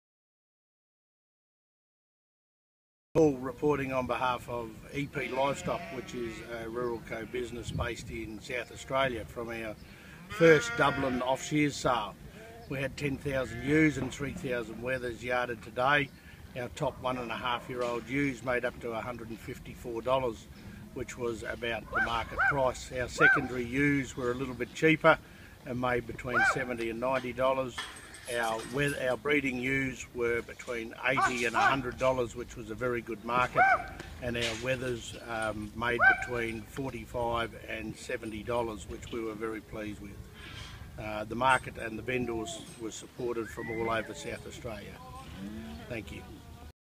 2013 09 26 Wayne Hall Dublin sheep sale comment
Wayne Hall reporting on the EP Livestock & Real Estate special sheep sale, Dublin SA on 26 Sept 2013.